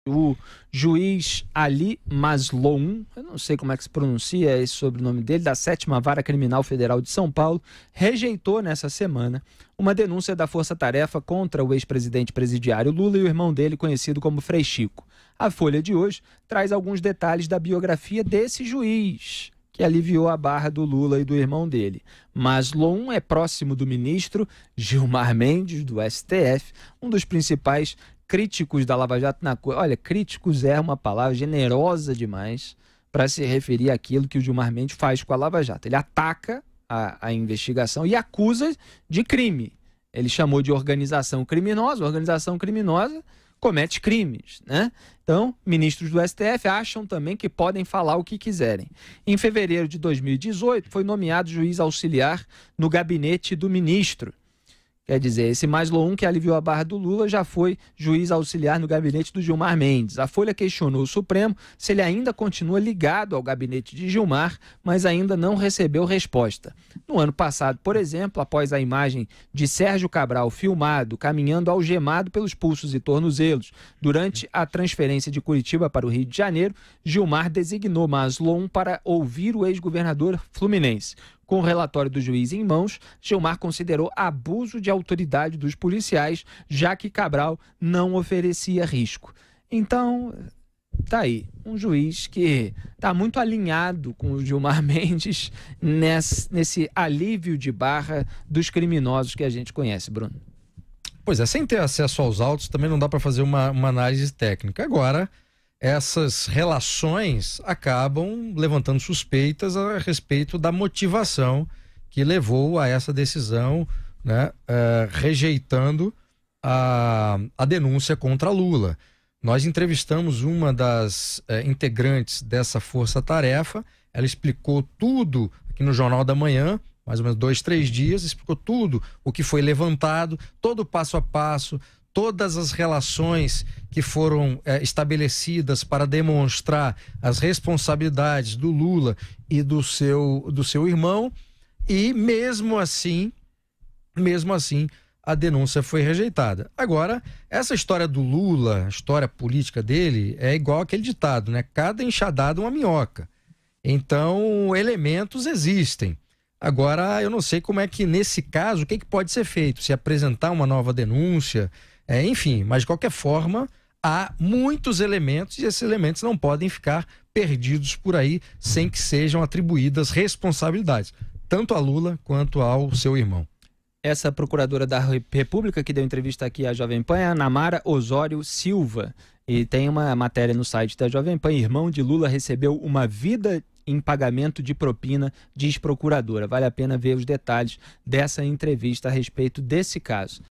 Juiz que aliviou a barra de Lula trabalhou para Gilmar
Felipe Moura Brasil, José Maria Trindade e Bruno Garschagen dão detalhes da biografia do juiz Ali Mazloum, da 7ª Vara Federal Criminal.